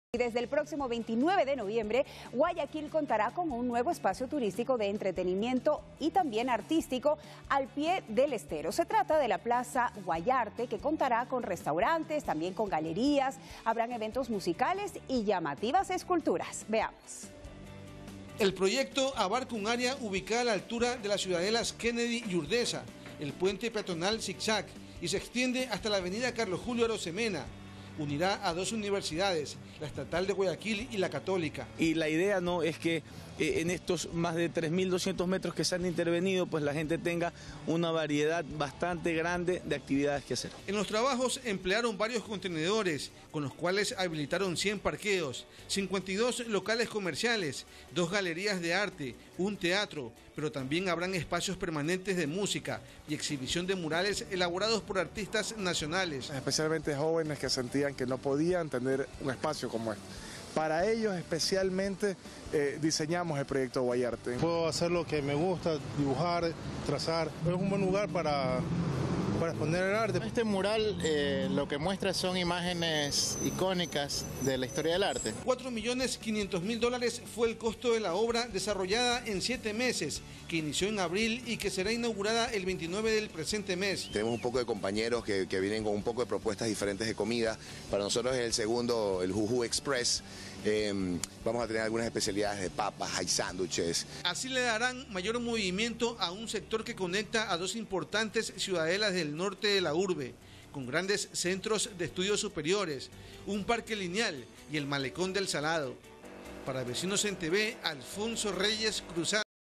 Plaza Guayarte se inaugura el 29 de noviembre en Guayaquil
Plaza Guayarte, un nuevo espacio turístico,  se inaugura el 29 de noviembre en Guayaquil.

Informa: Alfonso Reyes Cruzatty